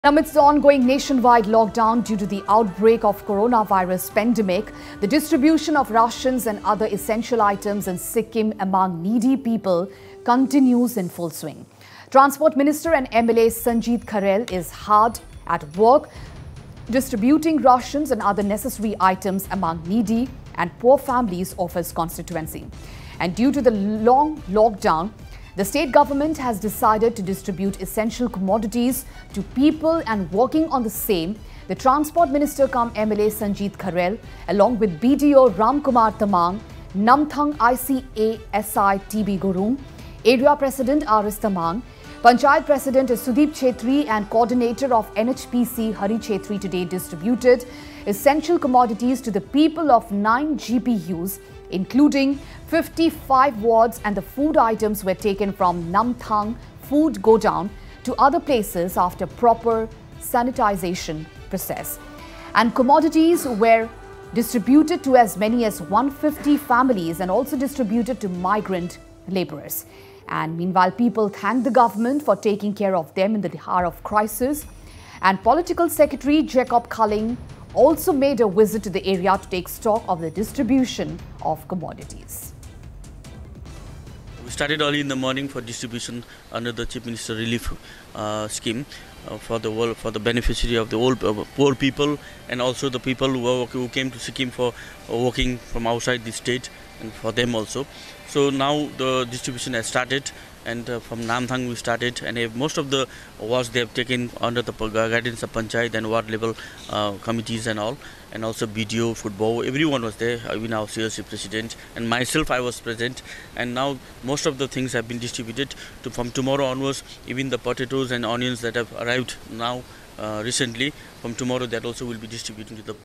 Sikkim: Ration, other essential items distributed among needy people in view of lockdown
Amidst the ongoing nationwide lockdown due to the outbreak of the Coronavirus pandemic, the distribution of rations and other essential items in Sikkim among needy people continues in full swing.
Transport Minister and MLA Sanjit Kharel is hard at work distributing rations and other necessary items among needy and poor families of his constituency.
Due to the long lockdown, the state government has decided to distribute essential commodities to people and working on the same the Transport Minister cum MLA Sanjit Kharel along with BDO Ram Kumar Tamang, Namthang ICASI TB Gurung, Area President RS Tamang, Panchayat President Sudip Chettri and Coordinator of NHPC Hari Chettri today distributed essential commodities to the people of 9 GPU’s including 55 wards and the food items were taken from Namthang Food Godown to other places after proper sanitation process.